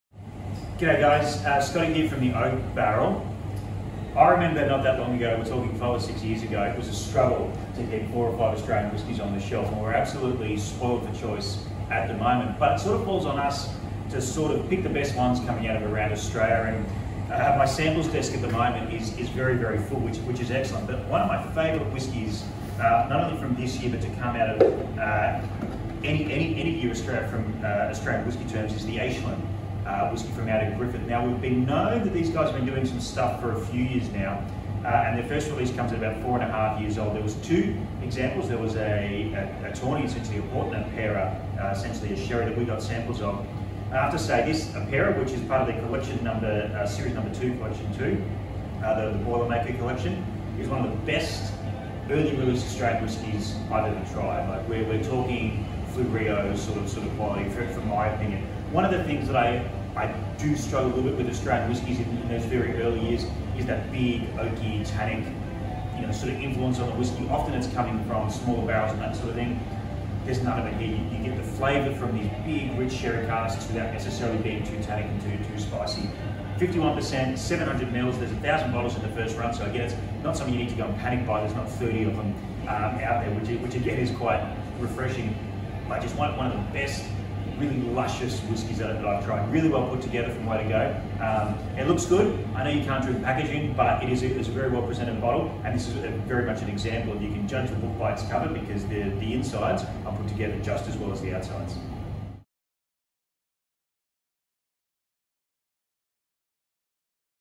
Aisling Distillery's Apera Australian Single Malt Whisky
One of the best early release whiskies from any Australian distillery in Scott's opinion.